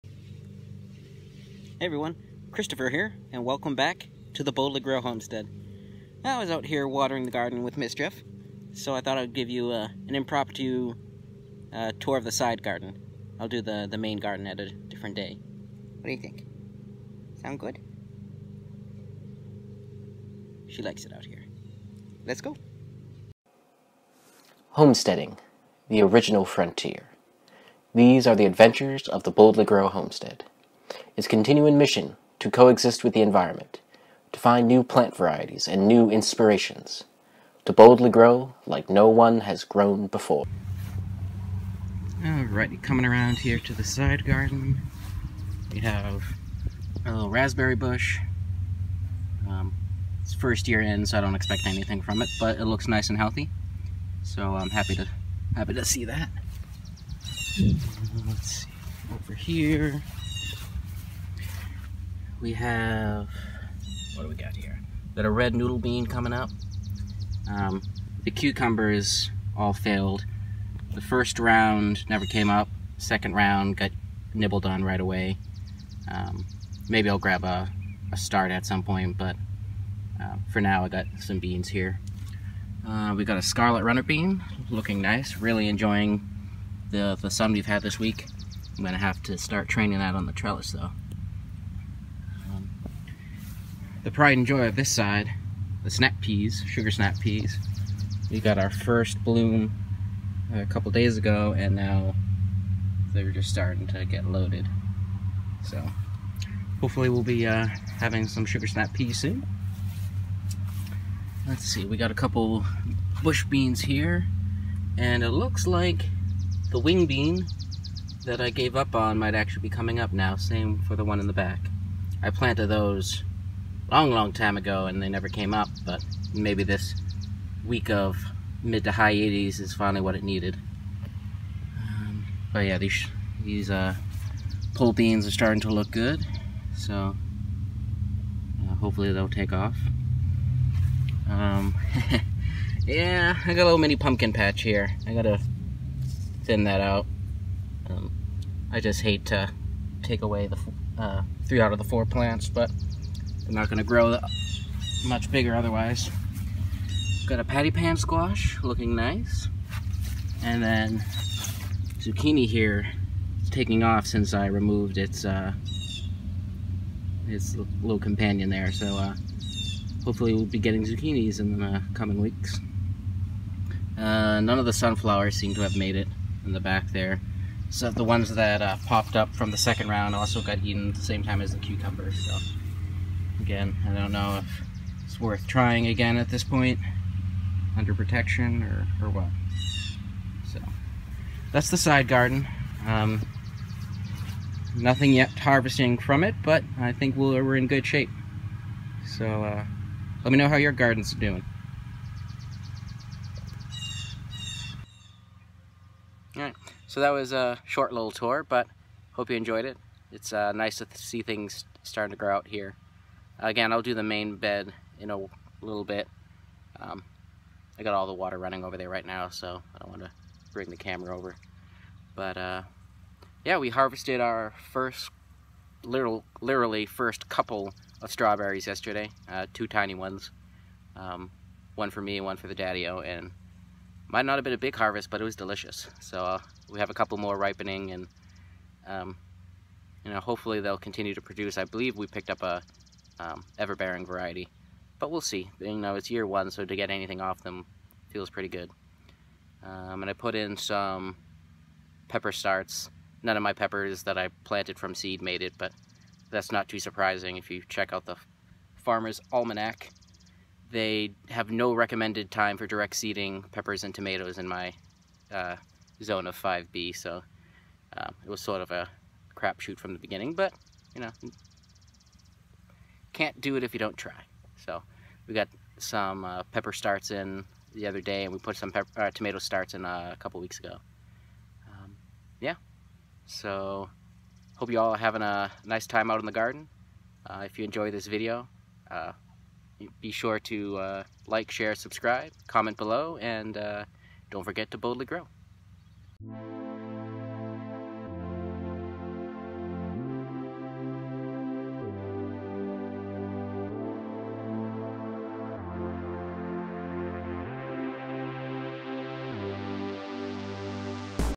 2019 Side Garden Tour - End of June
Welcome to the Boldly Grow Homestead in Upstate New York! Enjoy this short update of our side garden!
🌌RELEVANT LINKS🌌